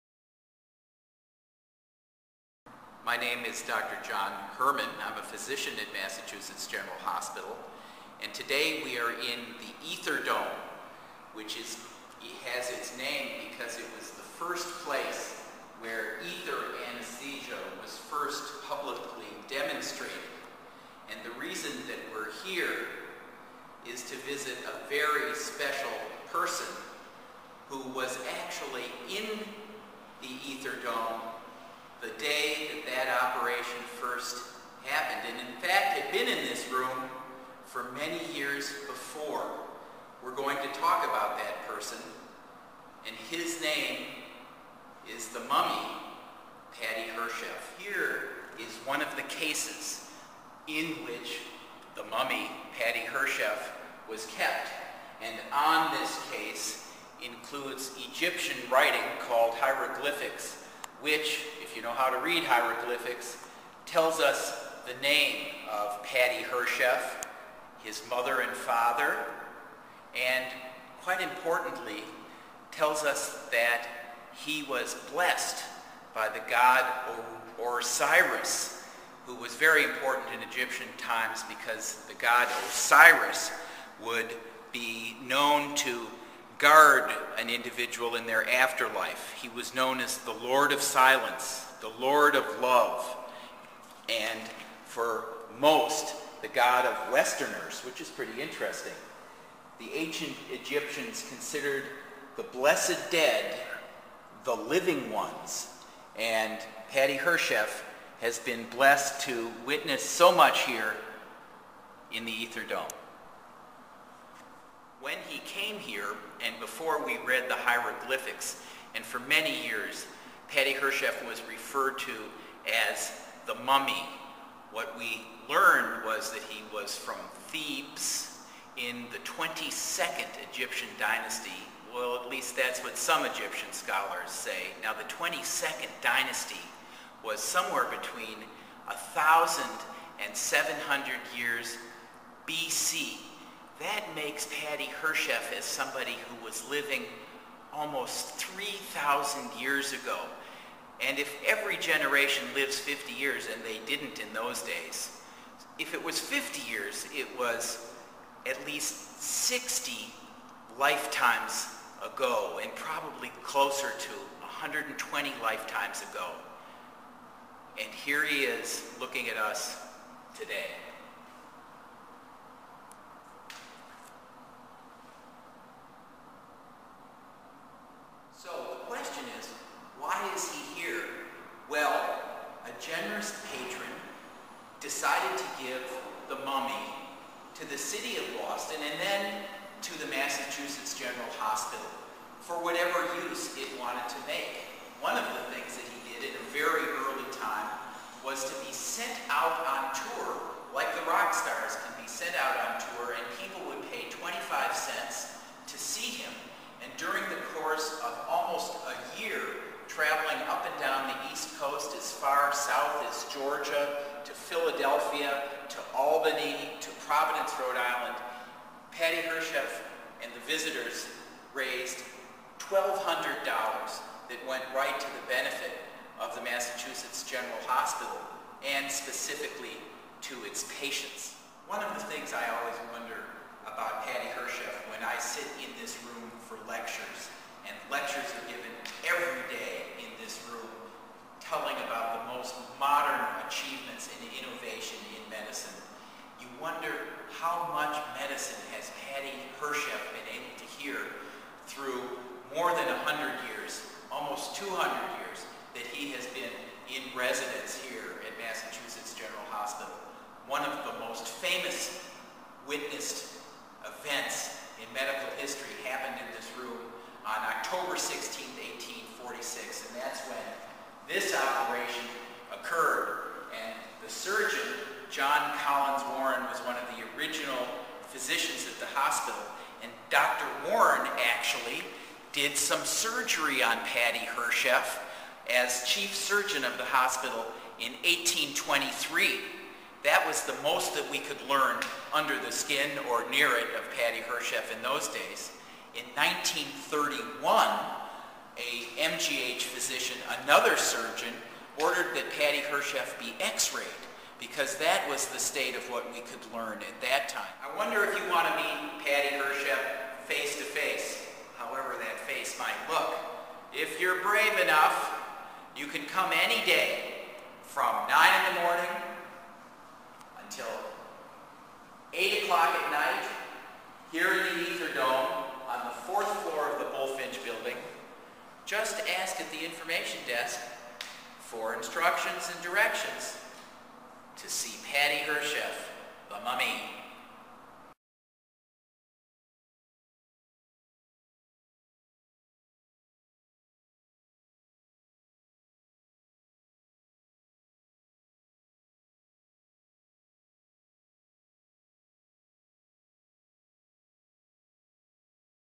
Padihershef: The MGH Mummy
Dr. John Herman of Massachusetts General Hospital provides a brief introduction to MGH's resident mummy: Padihershef. On display since 1824, Padihershef is available for viewing to the public each day from 8:00 a.m. - 9:00 p.m. in the Ether Dome.To learn more about Padihershef and MGH's history programs, connect with him on Facebook